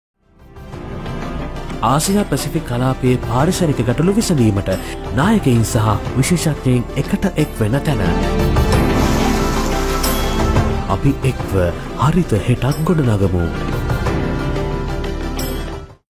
අපි එක්ව හරිත හෙටක් ගොඩ නගමු
රටවල් 39ක්
නියෝජිතයන් , නායකයන් , සහා විශේෂඥයින් 300කට අධික සංඛ්‍යාවක් සහභාගි වෙමින් දේශගුණික විපර්යාසවලට මුහුණ දිමටත්,ජෛව විවිධත්වය විනාශ විමට එරෙහිව සටන් කිරිමටත් ආසියා පැසිෆික් කලාපයේ දුෂණයට විසදුම් සෙවිමට කොළඹදි එක්වන වගයි